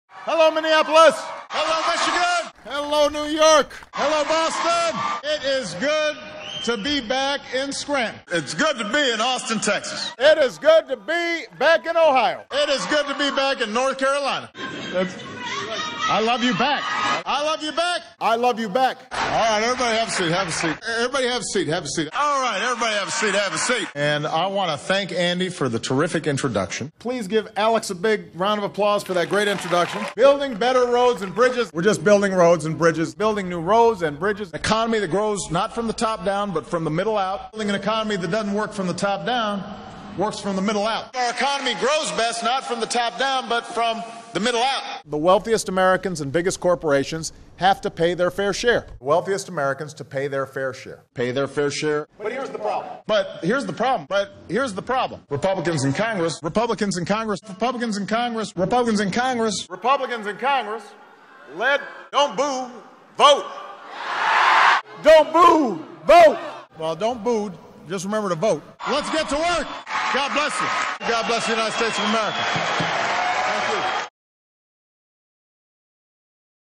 Every Obama Speech Ever | SuperCuts #74
Basically every speech that Obama gives.